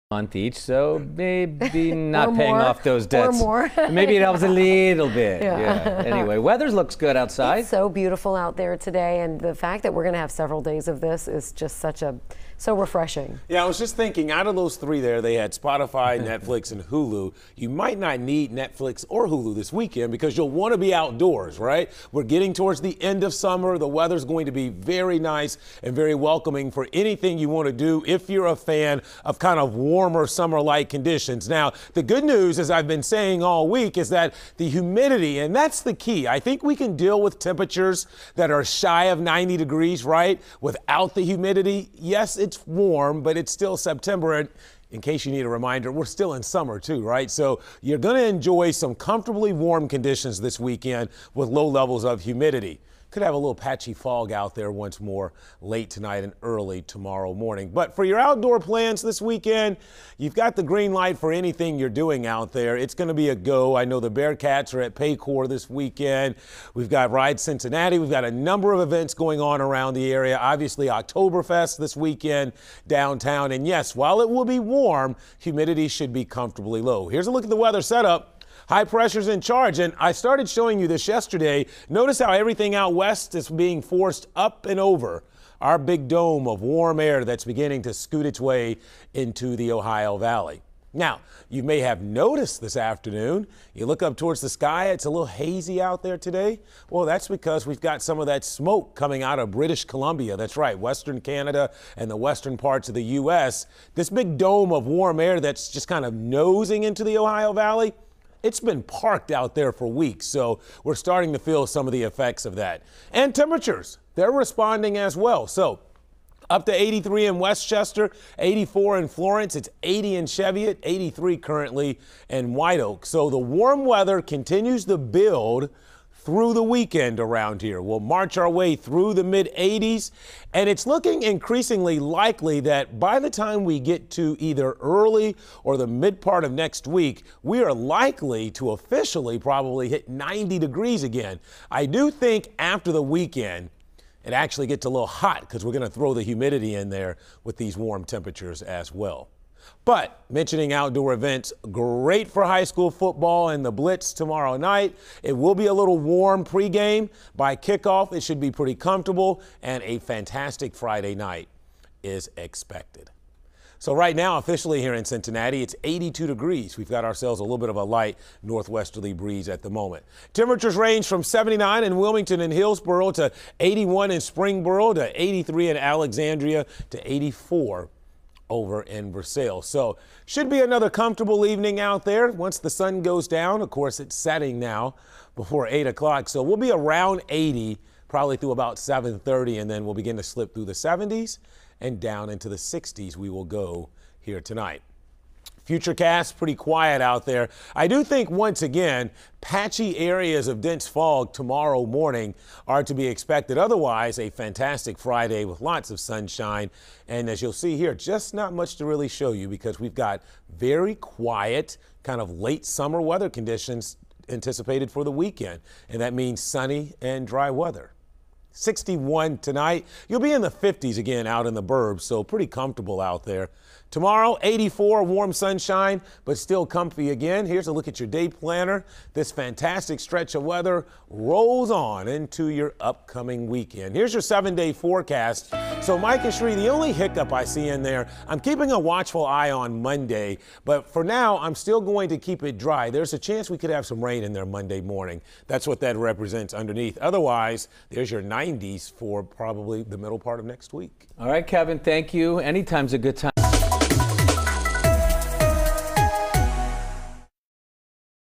Sunny and Warm Weekend
Sunny and warm for outdoor fun this weekend.